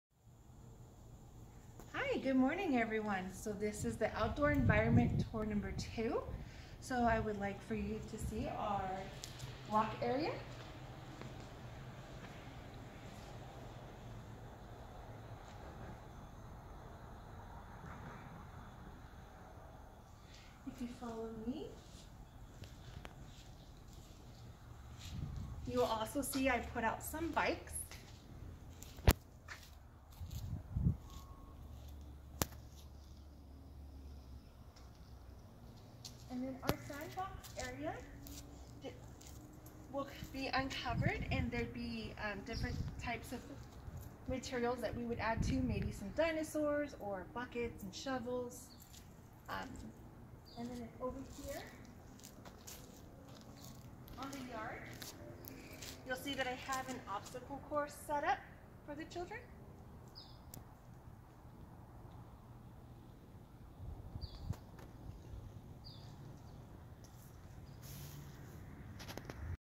Outdoor Classroom #2
Tour of the outdoor yard showing building, bikes, sand, and motor development